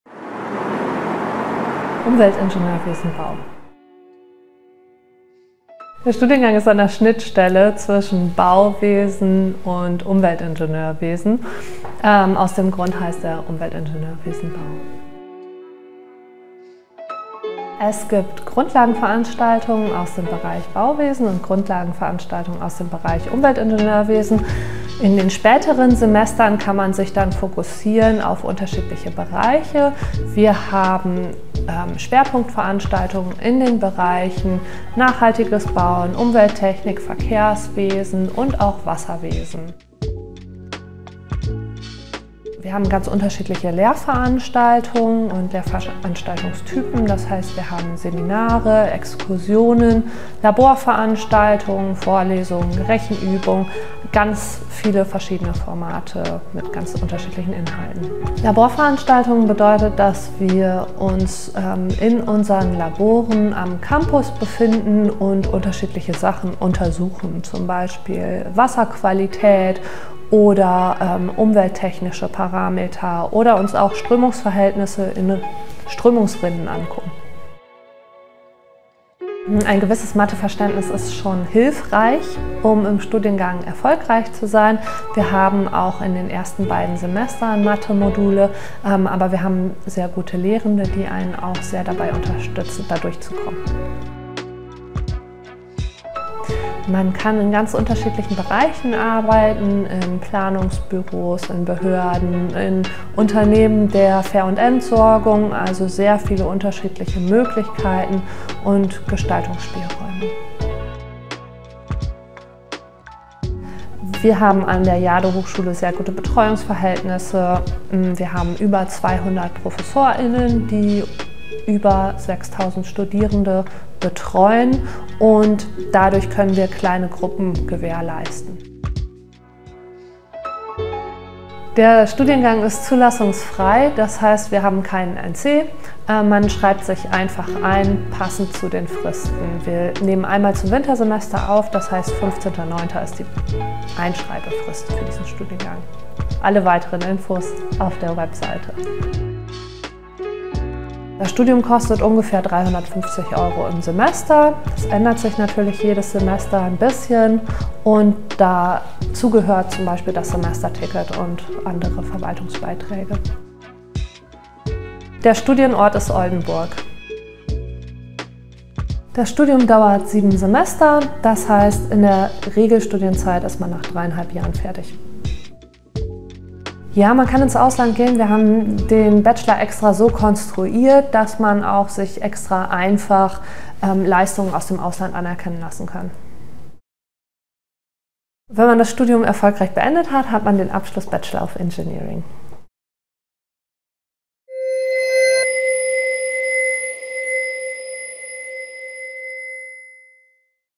FAQ zu Umweltingenieurwesen-Bau
Du interessierst dich für unseren neuen Studiengang Umweltingenieurwesen-Bau und hast noch einige Fragen? Dann ist dieses Video genau das Richtige für dich!